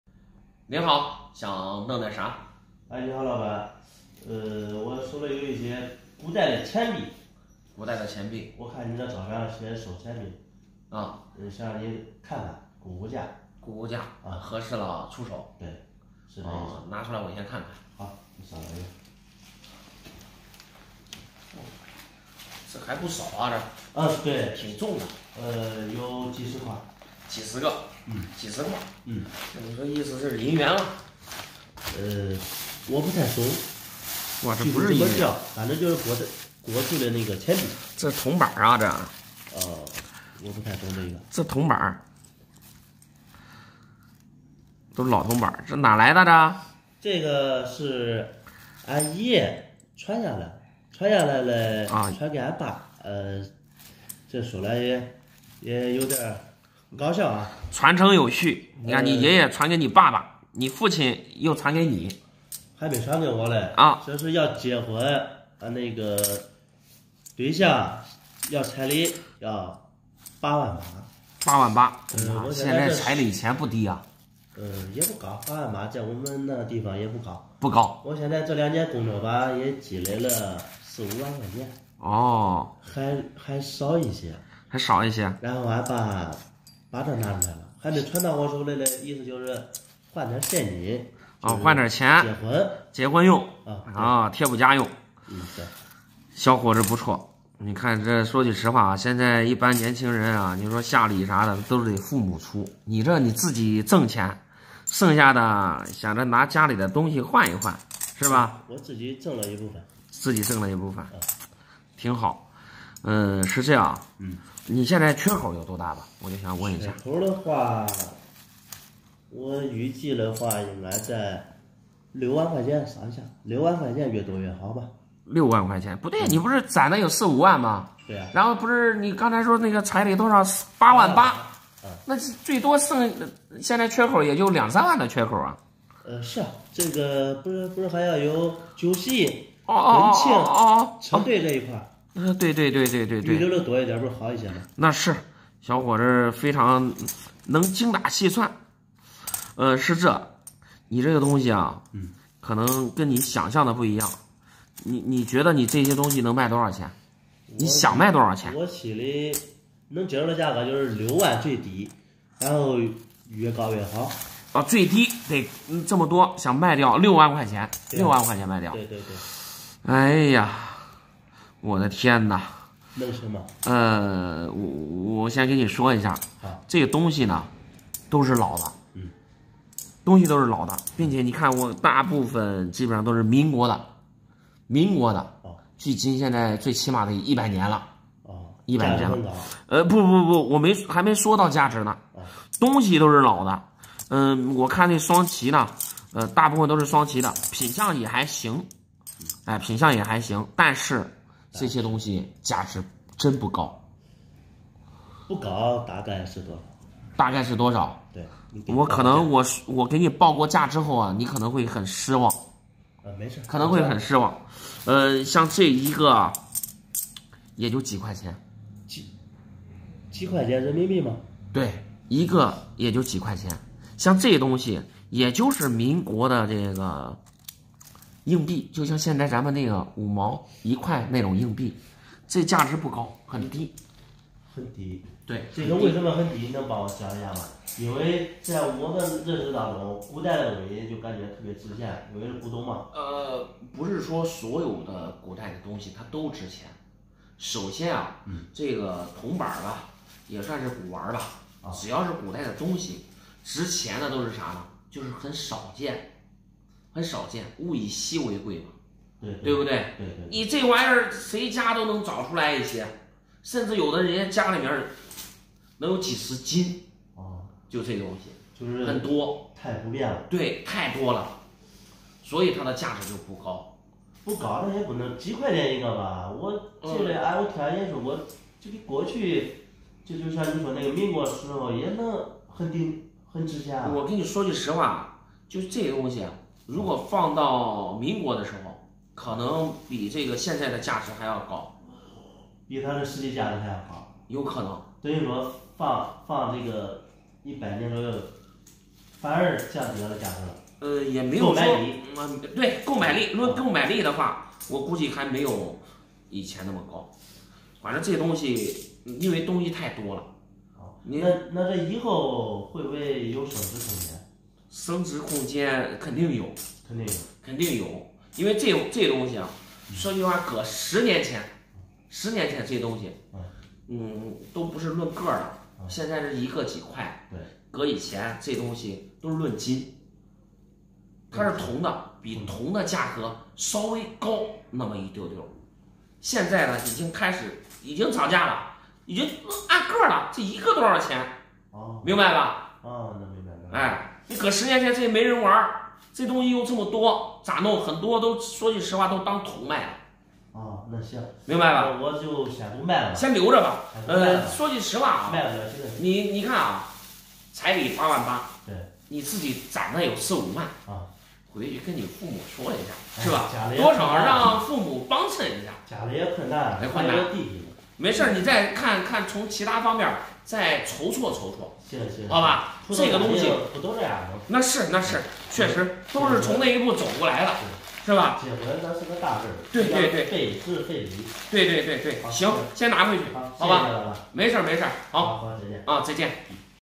为凑结婚彩礼出手家传铜钱，想着能值五六万，听到估价后失望了【小闫打铁镯】
请放心，本视频是一部原创拍摄的视频，没有任何不良行为。 一大包家传铜钱，想卖掉6万元，原来是为了结婚彩礼？